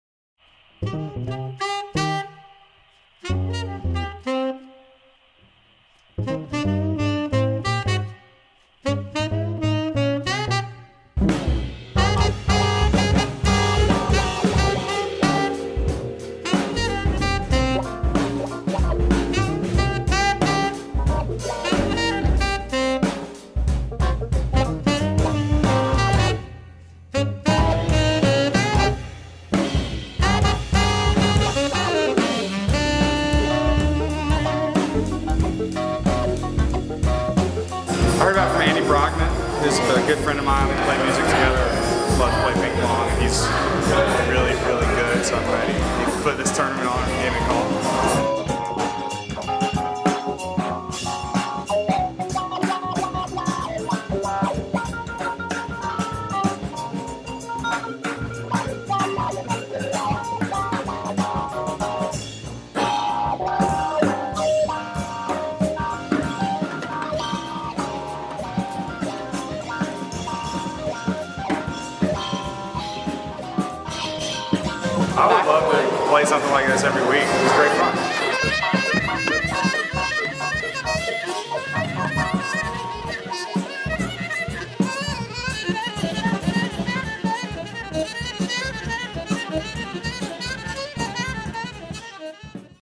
pongrva
1st Pong RVA event @ Yapple Yogurt in Carytown, Nov 4th.  Shot and edited by Donna Steele, music by Samson Trinh.